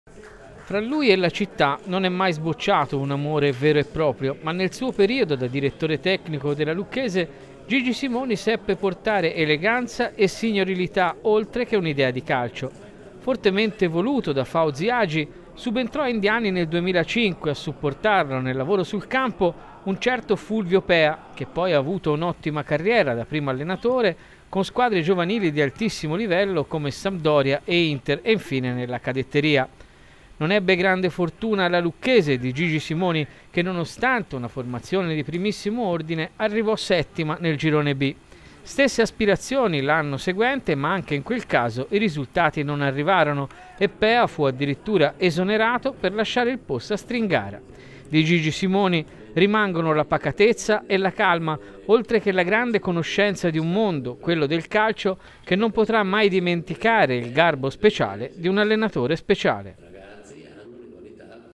Addio a Gigi Simoni
CALCIO - E' scomparso all'età di 81 anni gigi Simoni, uno degli allenatori italiani più conosciuti. Oltre ad aver allentato l'Inter nei tempi d'oro di Ronaldo, Simoni aveva diretto la squadra della Lucchese nella sfortunata annata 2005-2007